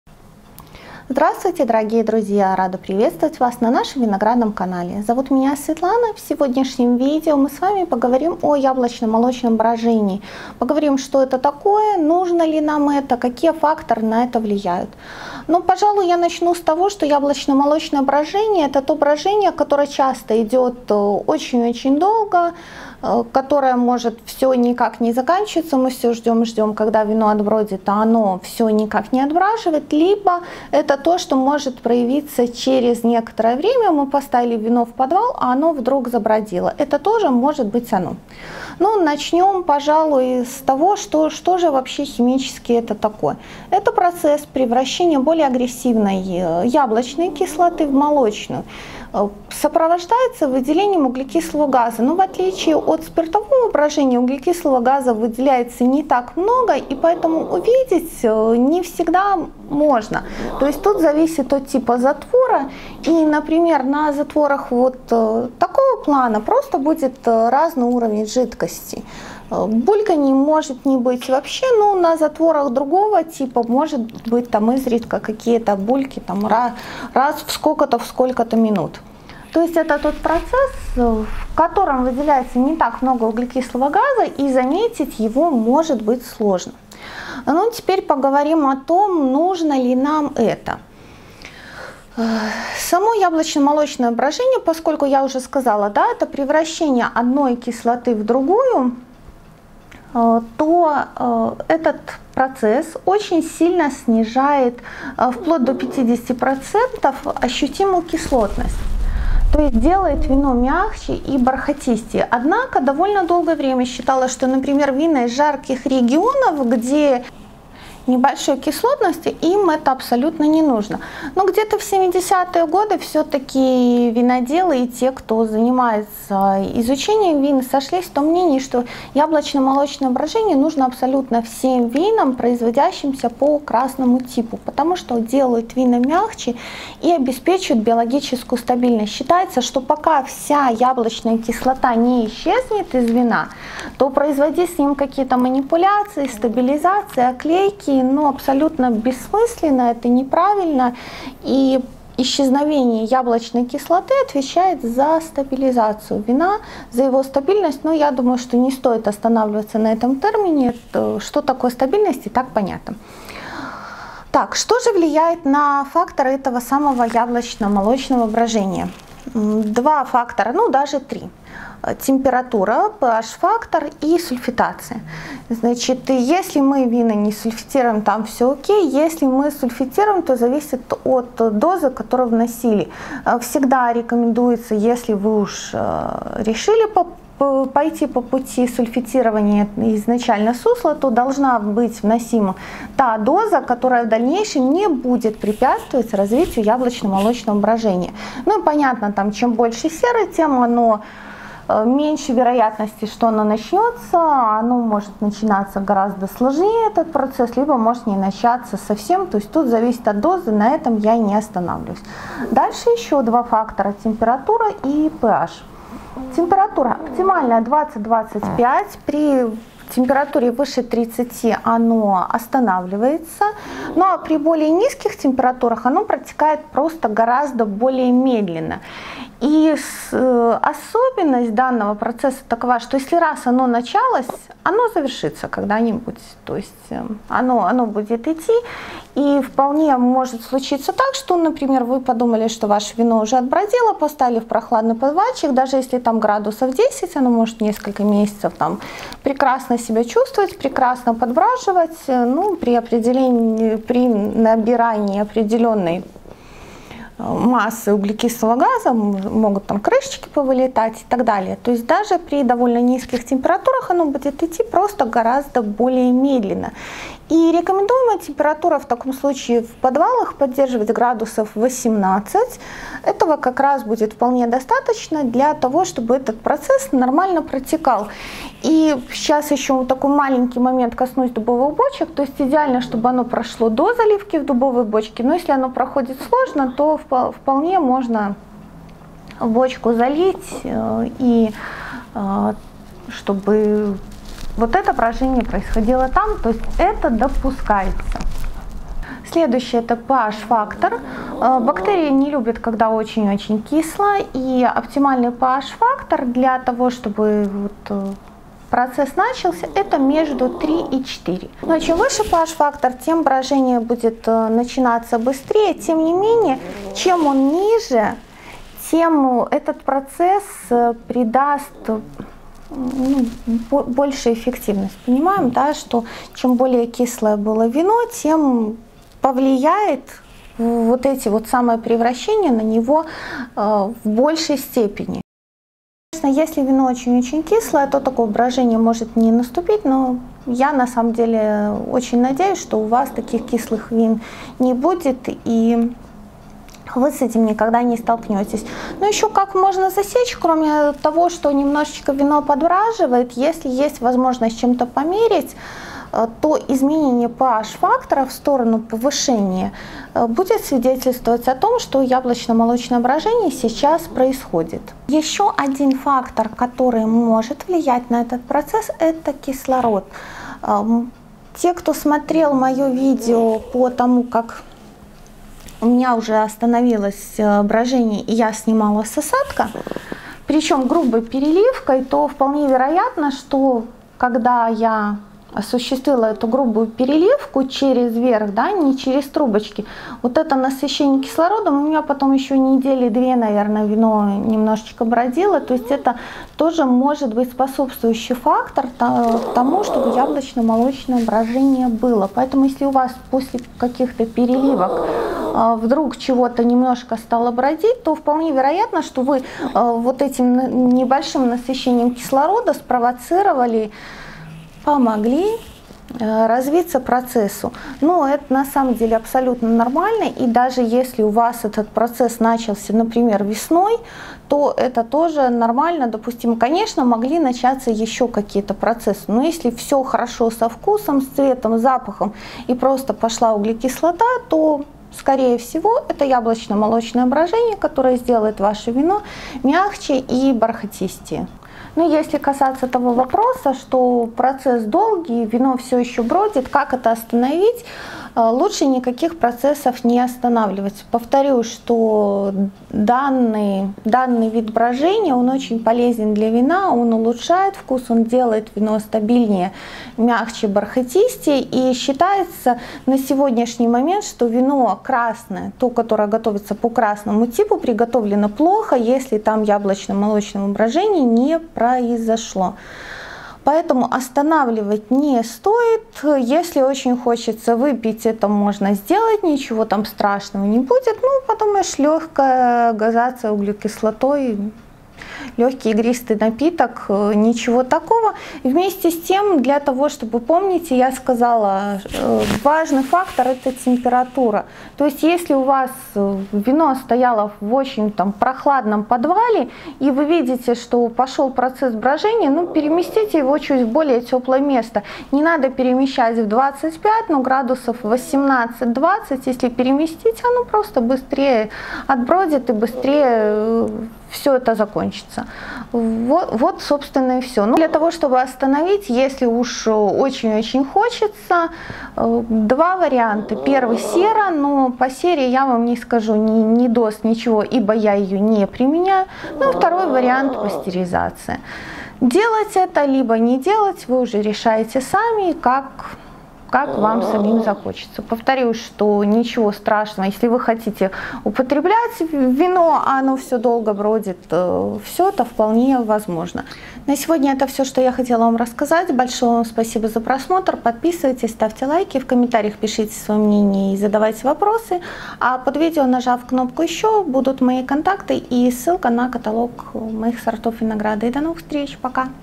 Яблочно-молочное брожение или брожение никак не заканчивается
В этом видео я рассказываю про яблочно-молочное брожение. Что это, нужно ли оно и какие факторы на него влияют.